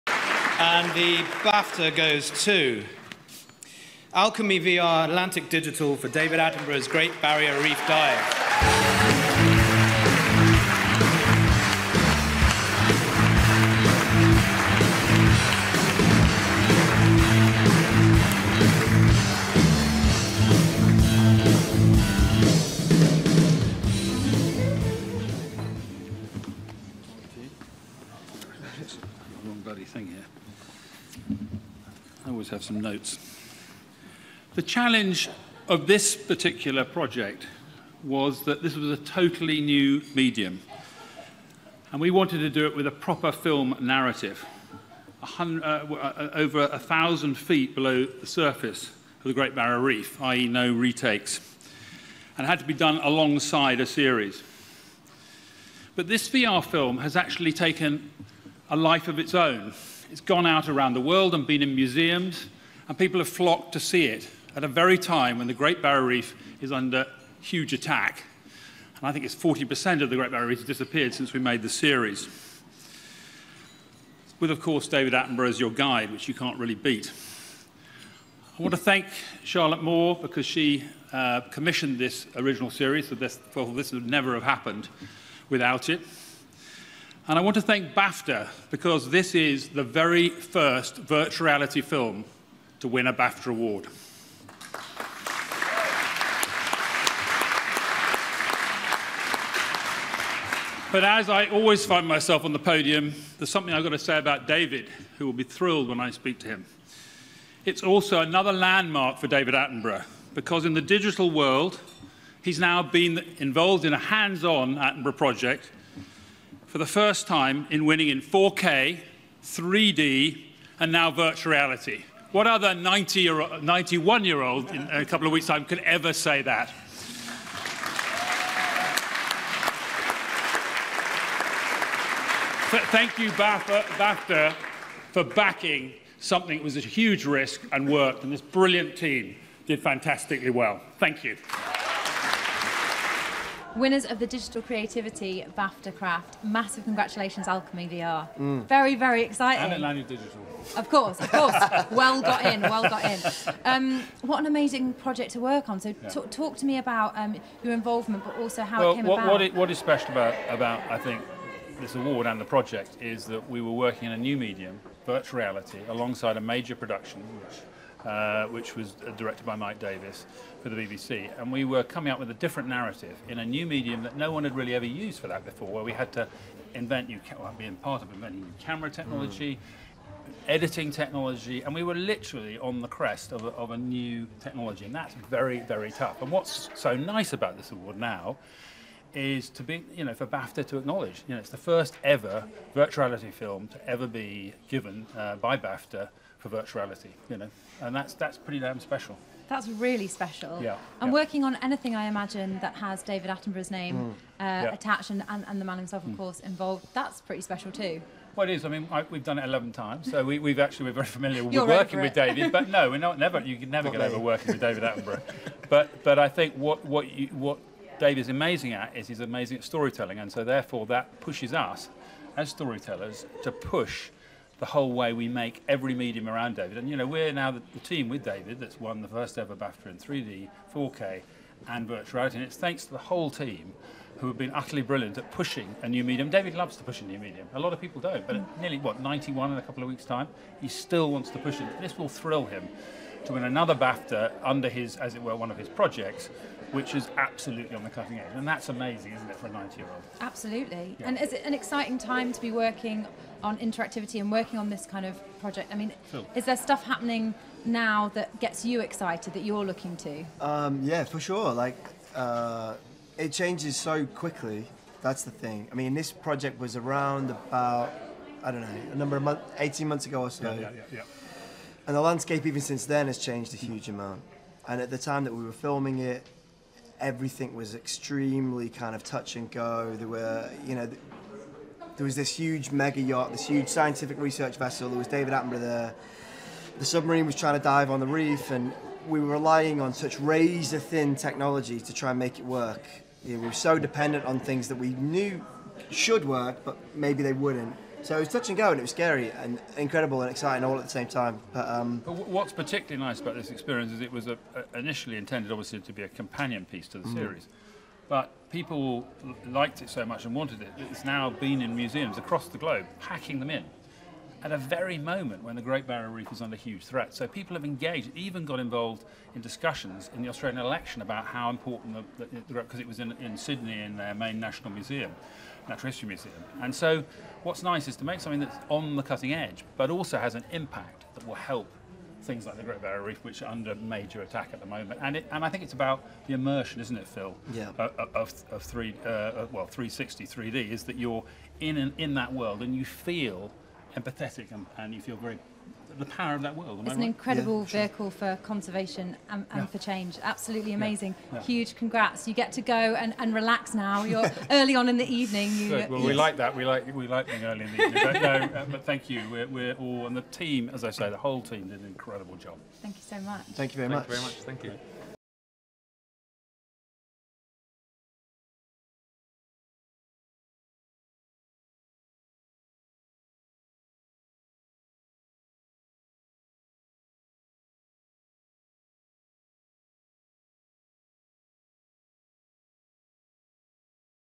Alchemy VR wins Digital Creativity for David Attenborough's Great Barrier Reef
The team from Alchemy VR wins Digital Creativity for David Attenborough's Barrier Reef at the British Academy Television Craft Awards 2017.